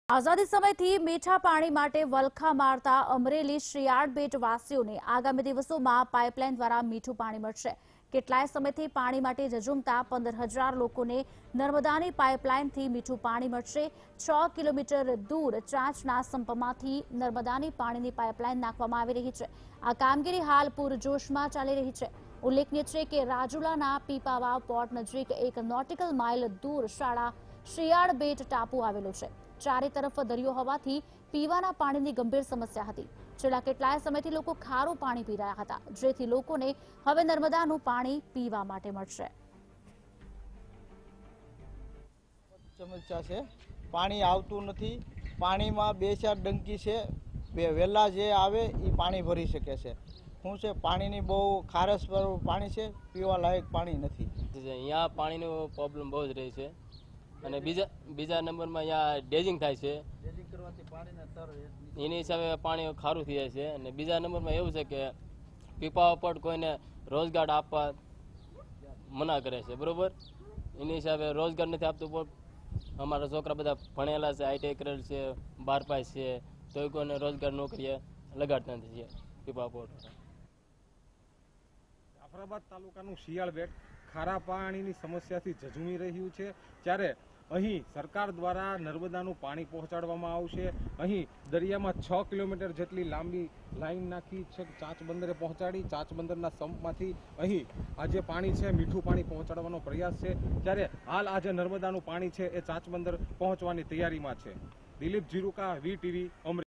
Amreli :People of Shiyal Bet to be get Drinking water of Narmada River After 60 Year | Vtv News
Amreli: People of Shiyal Bet to be get Drinking water of Narmada River After 60 Year.